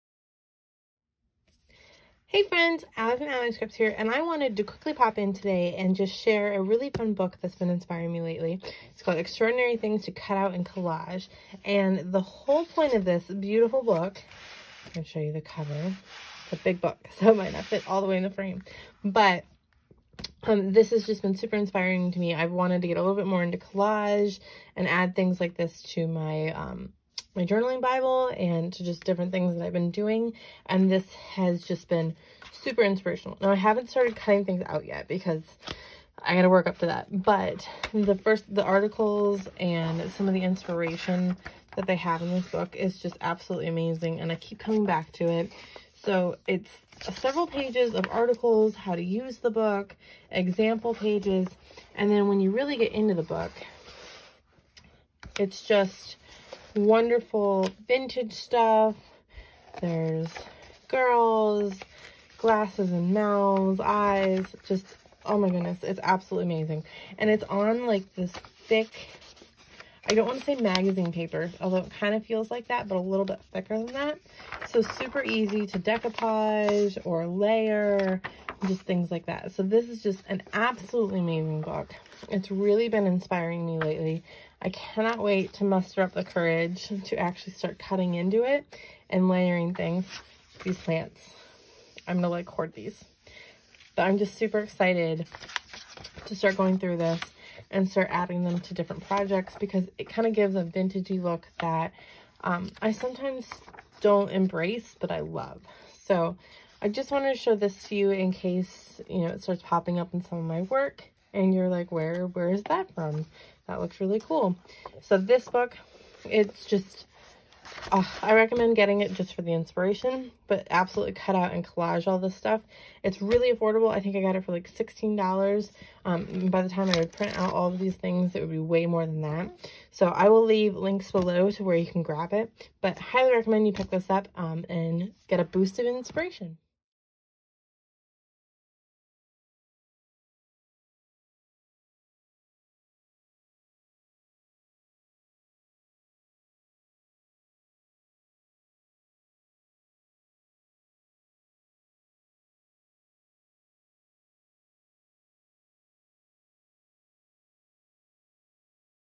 INSPIRED BY | Quick Flip Through of a New Collage Book!
Join Allie for a quick flip through of a fun new book that's been inspiring her lately! Check below for links!

Grab "Extraordinary Things To Cut Out And Collage"
from Amazon

My filming set-up:
Cameras:
Canon 60D
Canon Rebel
Filming arm